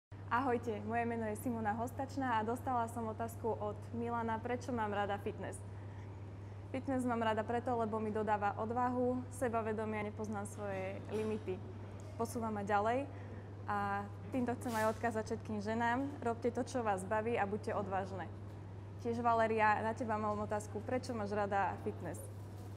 Simona Hostačná - fitness challenge
Join our fitness challenge 💪  and answer ❓ "WHY DO YOU LOVE FITNESS?" 💪✌️
Simona Hostačná answered this question 👍
Next ➡️  Valerija Slapnik 👍